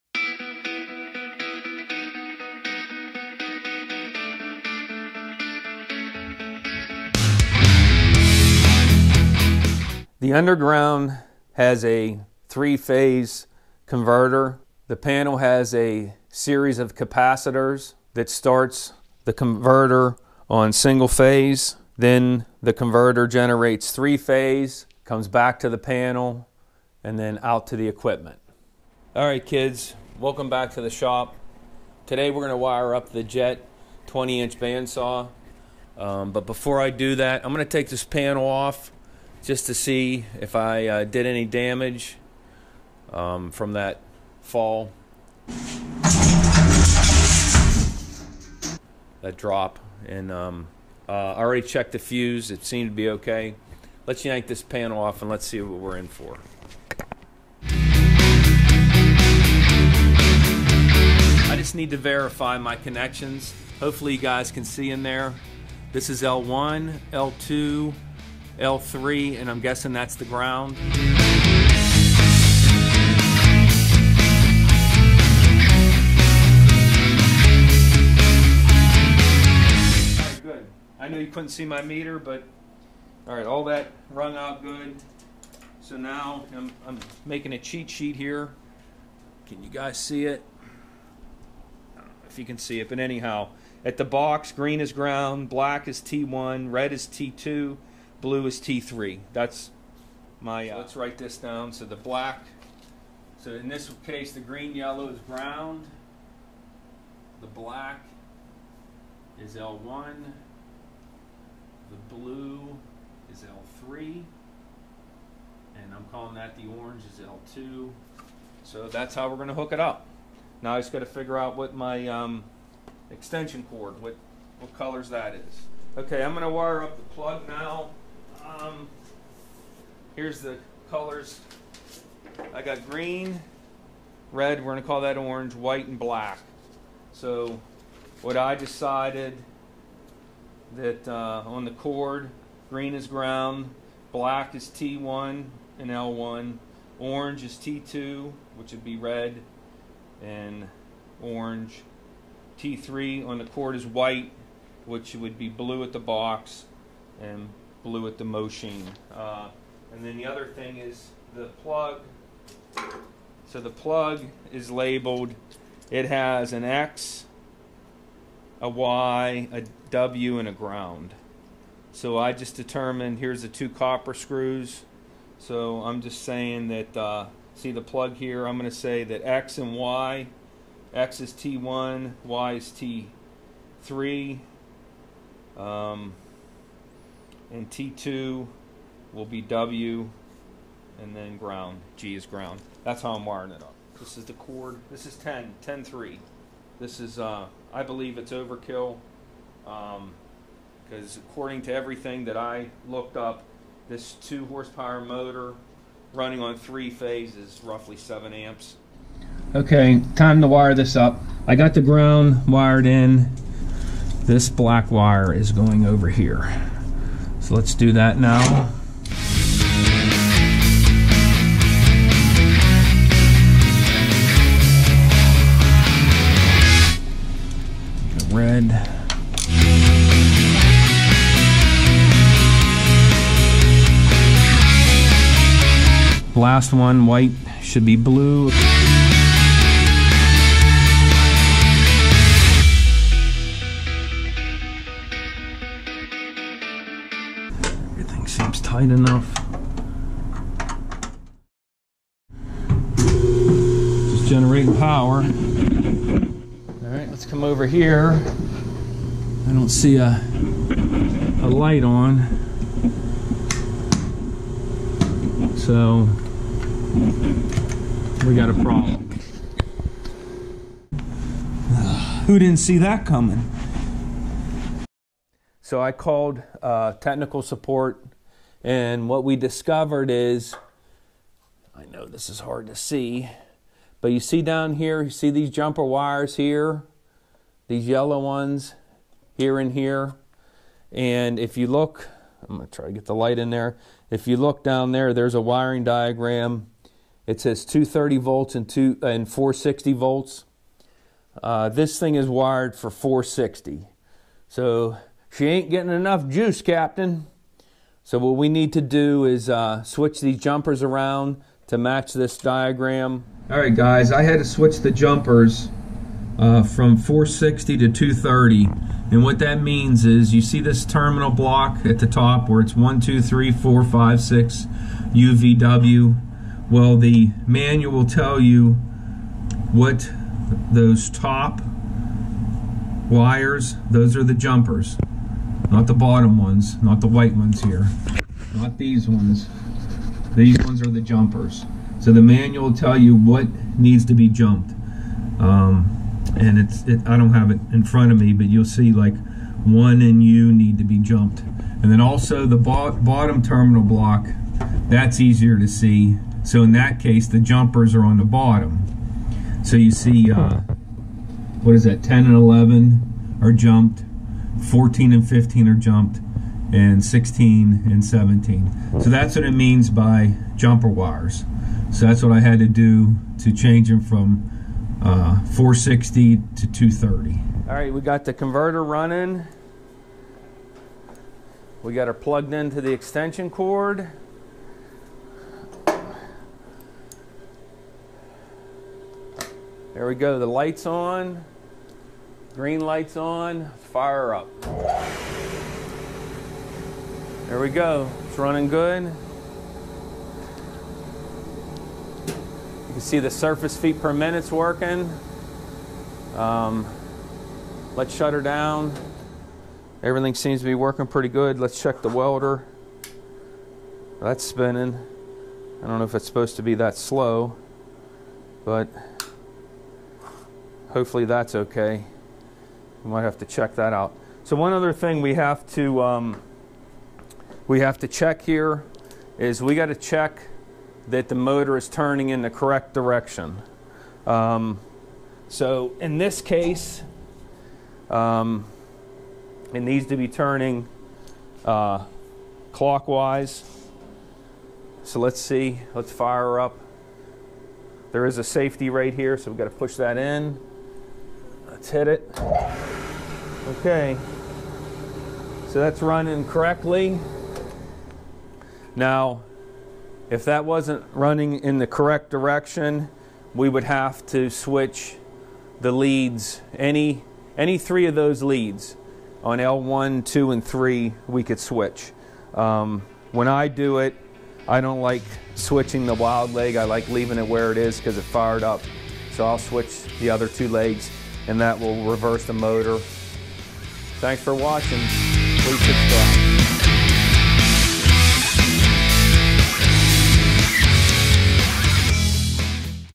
3 phase 2hp. Motor Wiring and Startup - Jet 20" Band Saw
I wire A 3 phase 2hp. Jet 20" Band Saw.